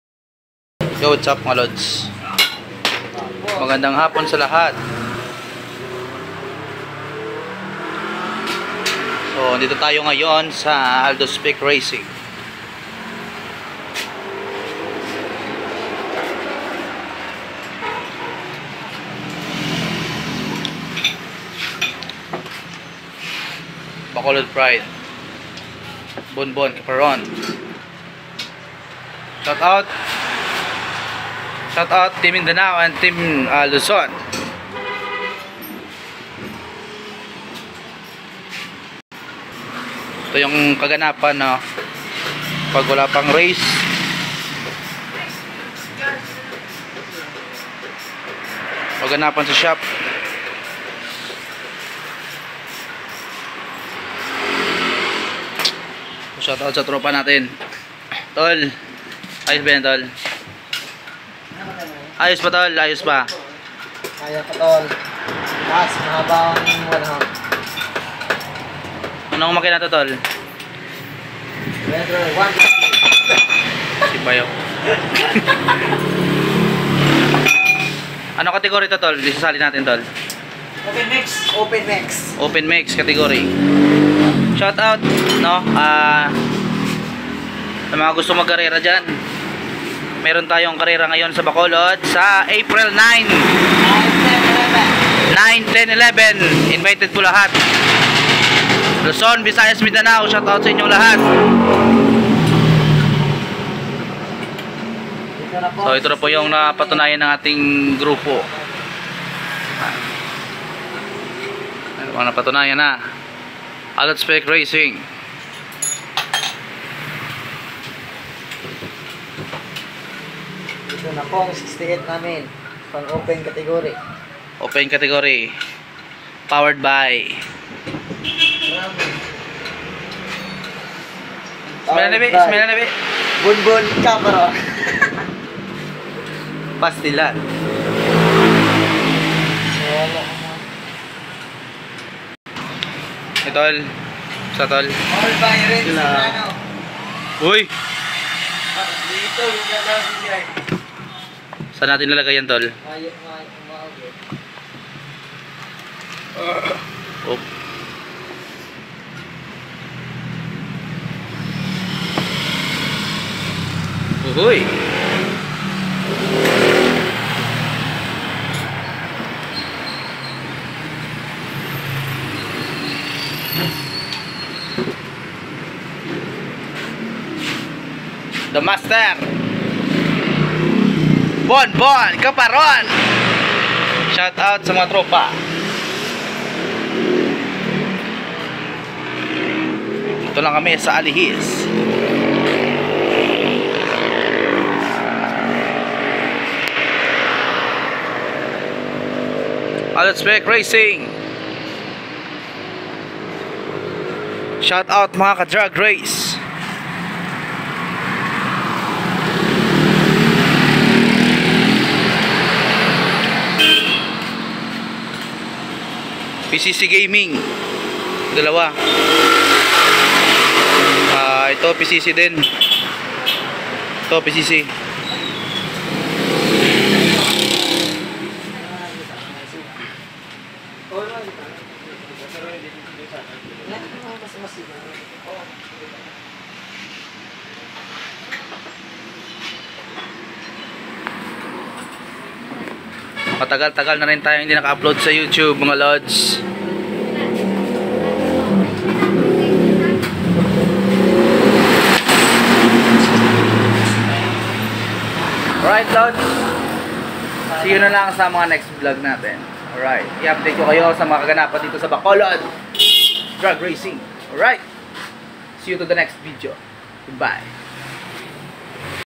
Aldotspecs Racing Team w/ Bacolod Pride (Bonbon Caparon)
shoutOut mga Lods presenting ALDOTSPECS RACING TEAM!
Abangan nyo po ang malahalimaw moves ng Caparon brothers 'Bonbon Caparon at Kirby Caparon'.